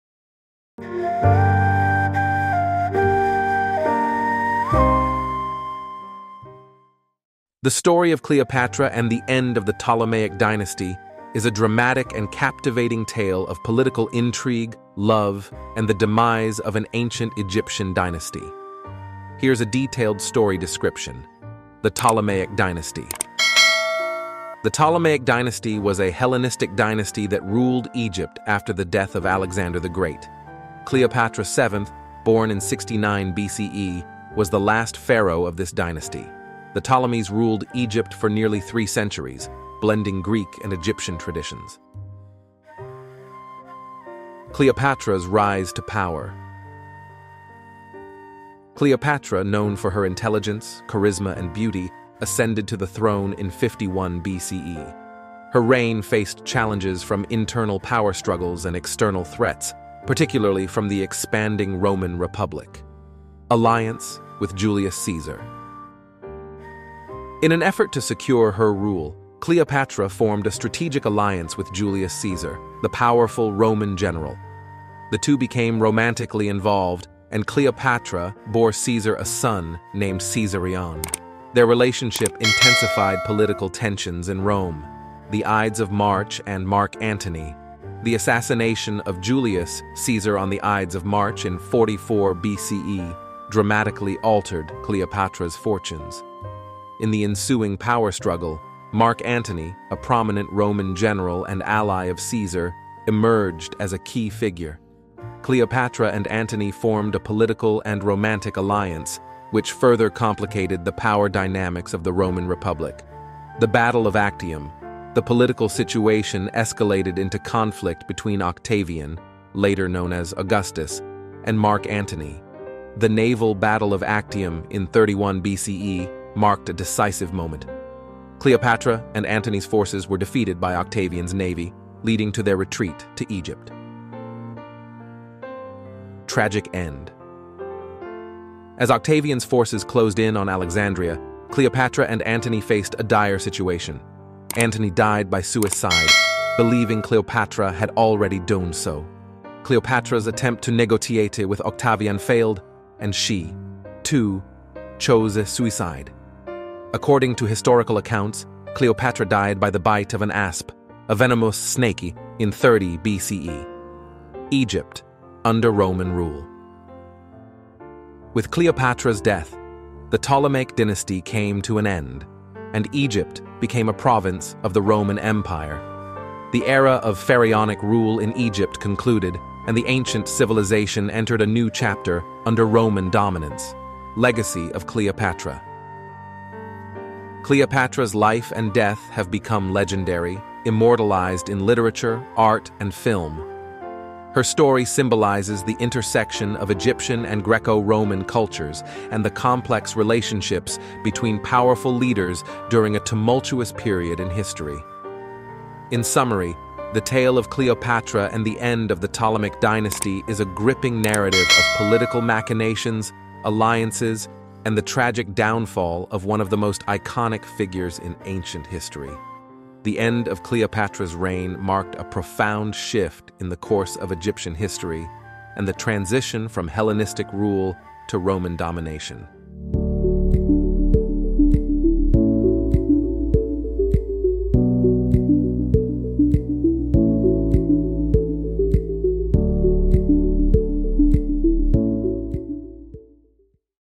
Cleopatra and the End of the Ptolemaic Dynasty | Unveiling the Final Chapter of Ancient Egypt
👑 Embark on a mesmerizing journey through time with "Cleopatra and the End of the Ptolemaic Dynasty," a captivating exploration that delves into the final chapter of Ancient Egypt's illustrious history. 🏛️ Join us as we unravel the enigmatic life of Cleopatra and witness the dramatic events that led to the demise of the Ptolemaic Dynasty.

🔑 Key Features:

Cleopatra's Life Unveiled: Explore the life, loves, and intrigues of one of history's most iconic figures, Cleopatra, Queen of the Nile.
Dramatic Events Leading to Decline: Witness the political machinations, conflicts, and alliances that marked the decline of the Ptolemaic Dynasty.
Legacy and Impact: Delve into Cleopatra's lasting influence on Egyptian history and her role in the transition from ancient empires to the dawn of a new era.
🏺 Featured Episodes:

"Cleopatra: Queen of Intrigue and Elegance" 👑
"The Fall of Alexandria: Twilight of the Ptolemaic Dynasty" 🌅
"Legacy of the Nile: Cleopatra's Enduring Impact" 🌊
Let's unveil the mysteries of Ancient Egypt and witness the dramatic conclusion of a legendary dynasty! 🏛️🔔